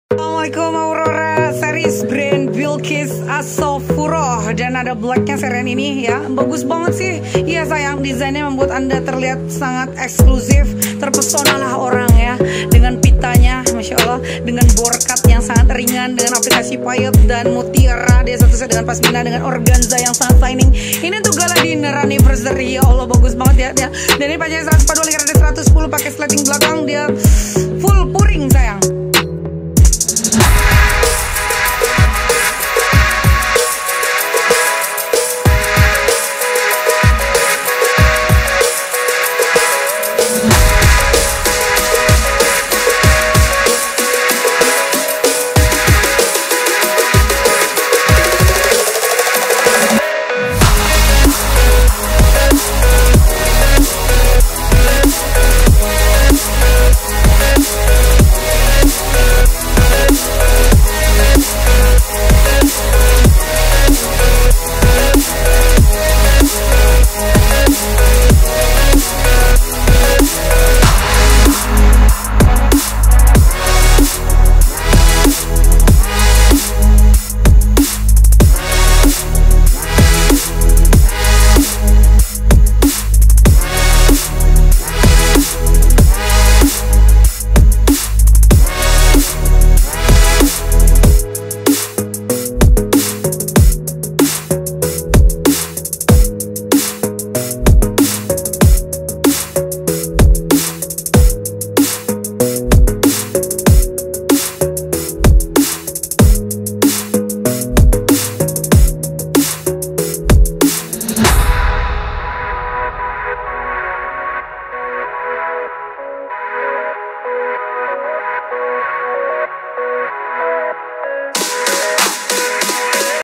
AURORA SERIES PART 2
Bismillah...
ready!!

Aurora series
(Dress + pashmina payet

Ld 110cm
Pb 142cm
Mat.babydoll premium mix organza+brukat import,lapis puring hg
Aplikasi payet import
Sleting belakang
Karet belakang
Wudhu friendy

*harga 435